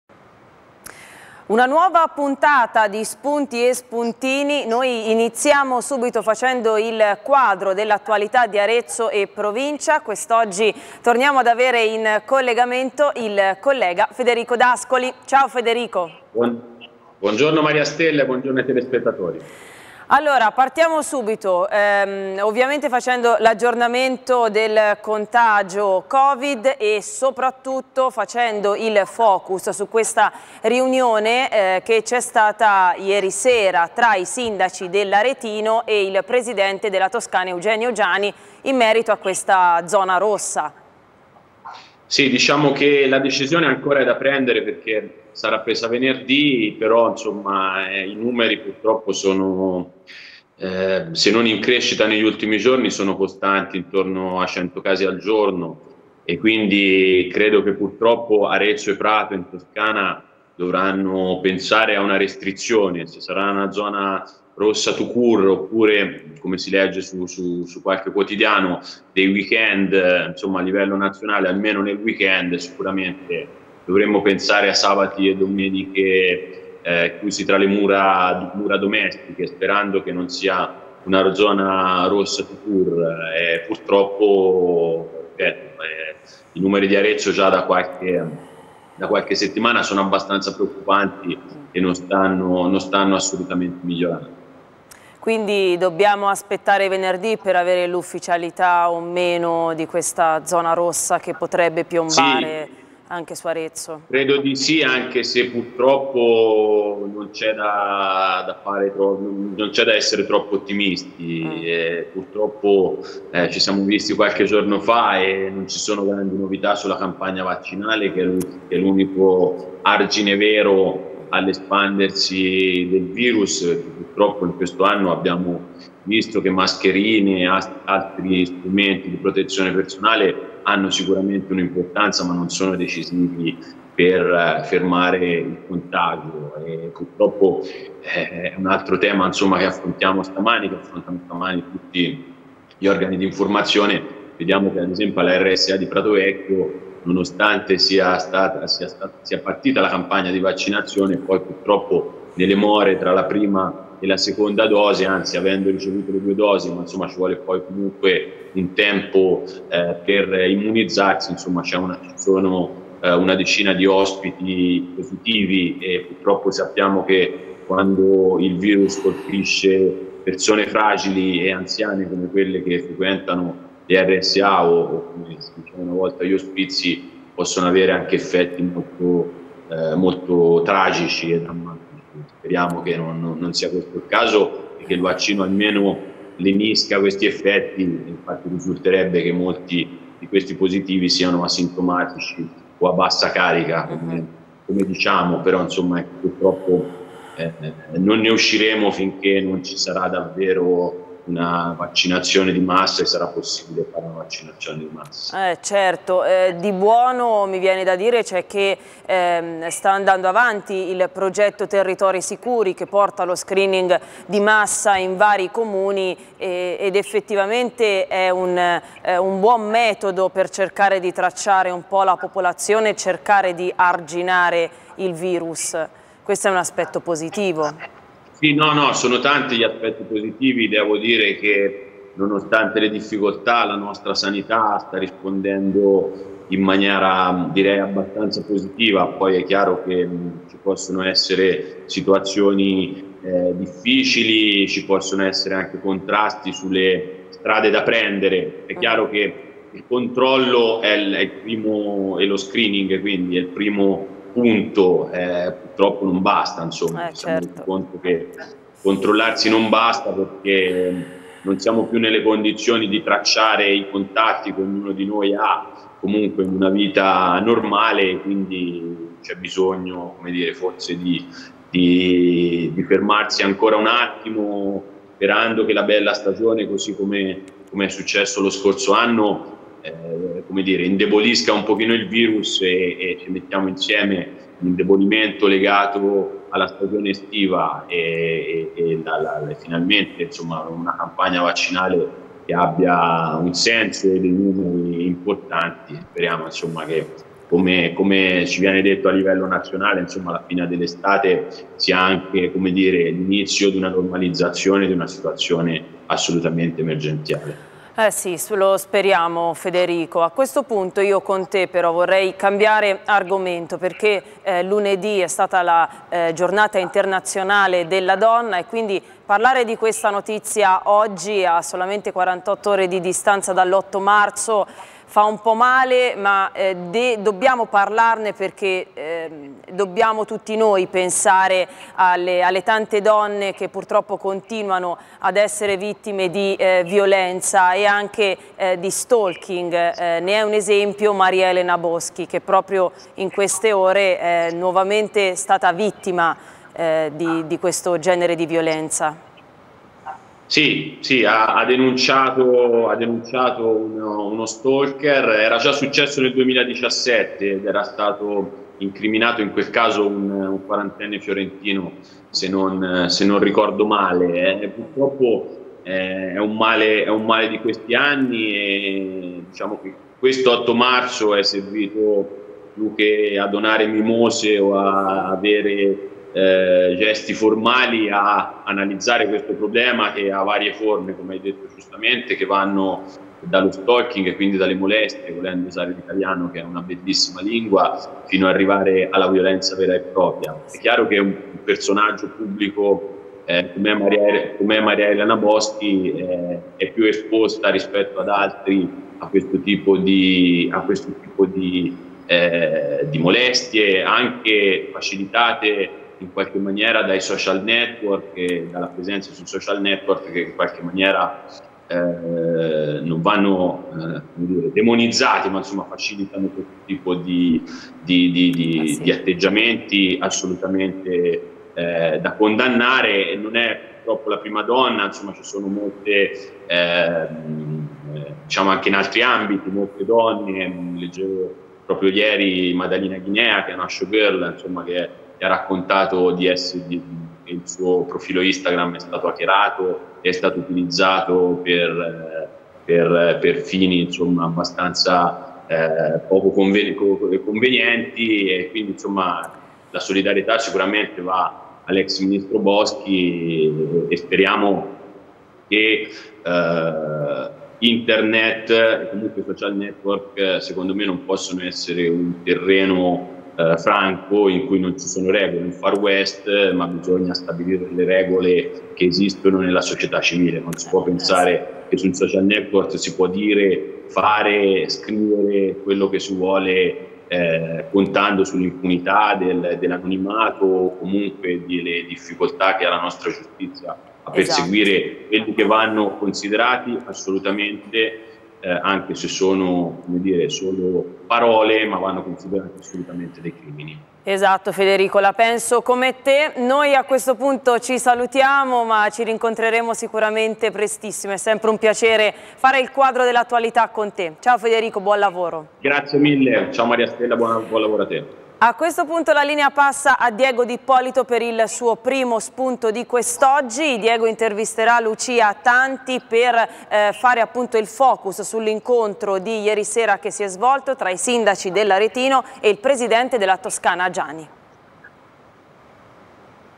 Spunto & Spuntini: "Una finestra sull'attualità" con Federico D'Ascoli
Federico D'Ascoli ospite nello Spunto di Maria Stella Bianco